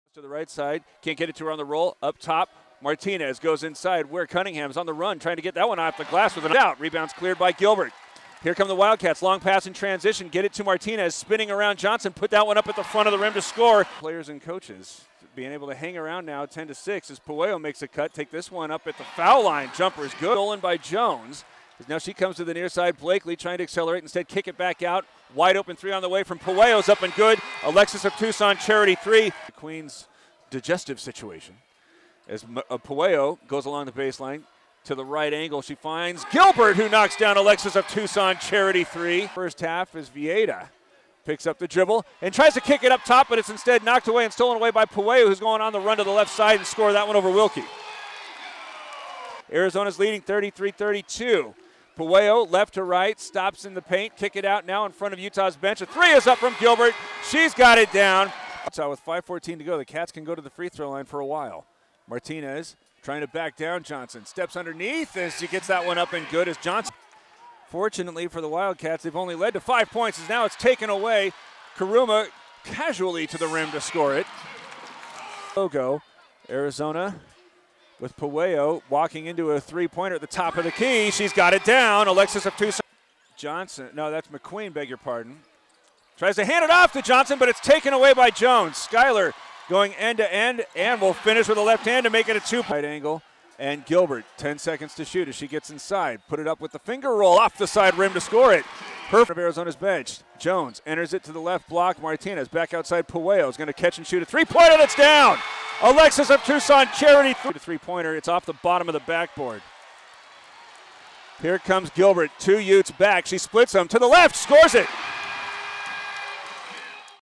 Arizona Stuns No. 15 Utah in Overtime
The Wildcats take the No. 15 Utes to OT in McKale and pull out the win.